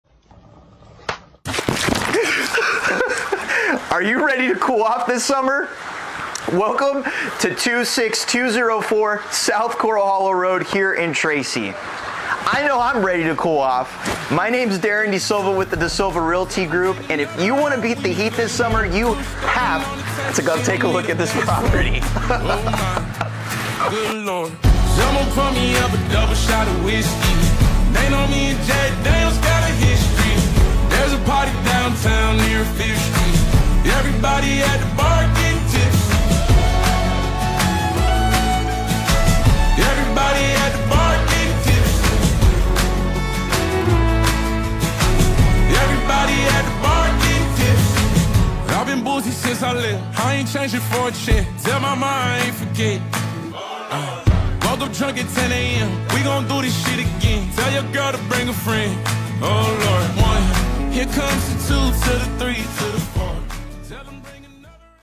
JUST LISTED! 📍26204 S Coral Hollow rd.
🏡 Listed at $1,200,000 Schedule a private tour with me to show you this listing!

Who do you know looking to buy or sell real estate?

I am NEVER too busy for your referrals!